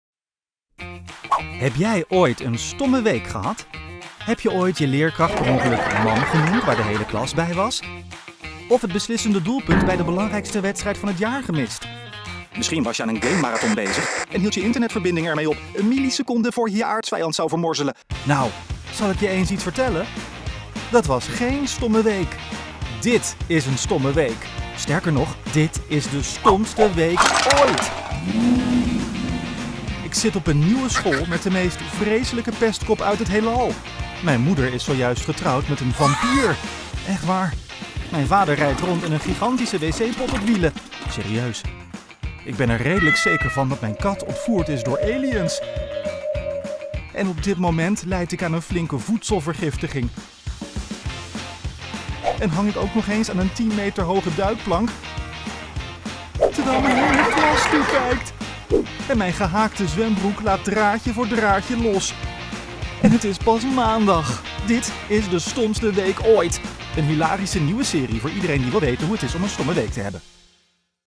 Stomste week ooit Boektrailer
Trailer van het boek Stomste week ooit ! ingesproken door Kevin Hassing